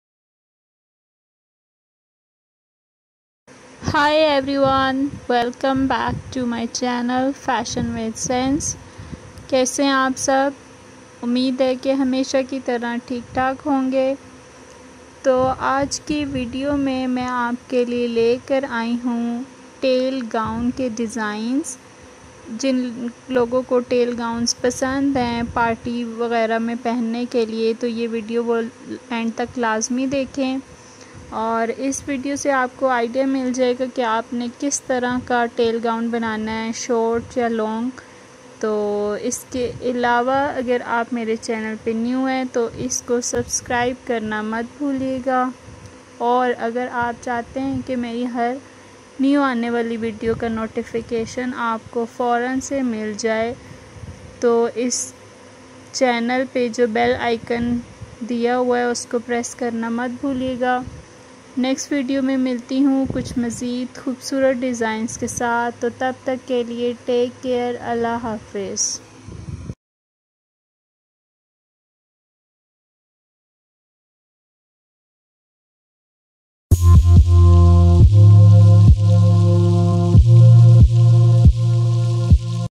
BEAUTIFUL TAIL GOWN DESIGNS FOR GIRLS | LATEST DESIGNS | FASHION WITH SENSE
Hi Everyone!

This video is about the most beautiful and latest tail gown designs for girls.